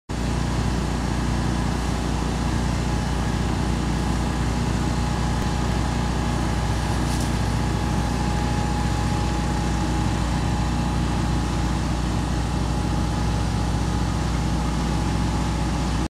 Spraying For Gypsy Moth Caterpillars
Our Greener Solutions Division live footage of spraying for the Gypsy Moth Caterpillar.